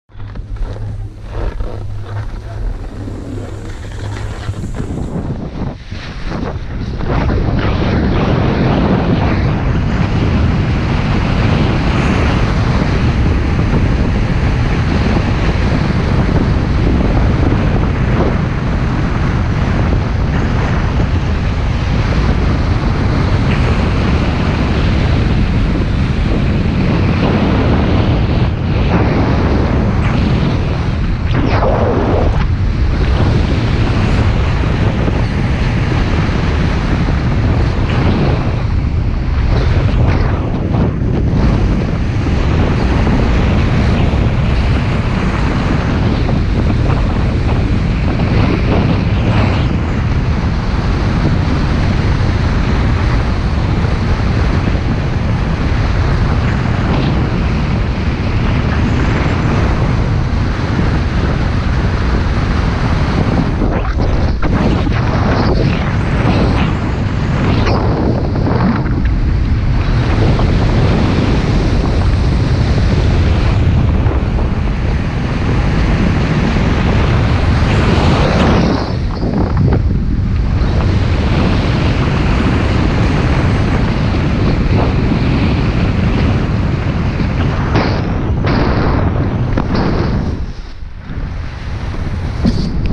Touching the white
My second Season in Speedriding. Getting confident with Skies. =)
Favorable conditions in Tyrol - Austria
Short run but very efficient.